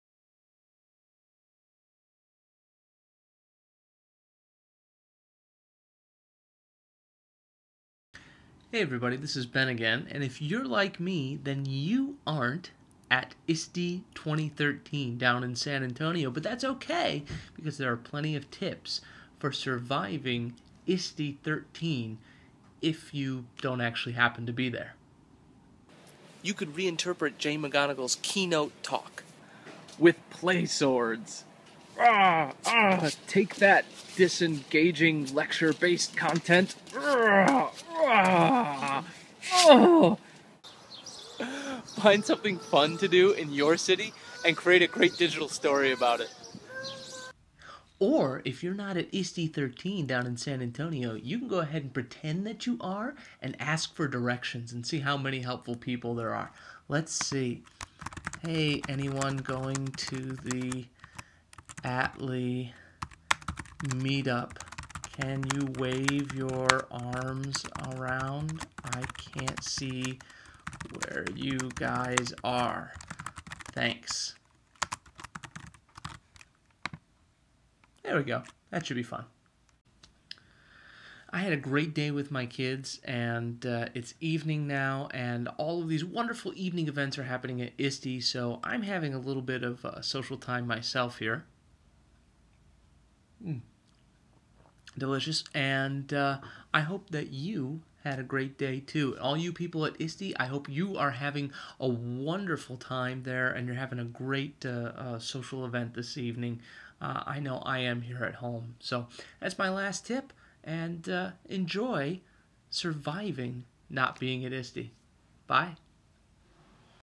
More Tips for Surviving ISTE13 If You're Not There
Not at ISTE13 in San Antonio? No problem! Here's a few more tips for surviving the conference if you're not there!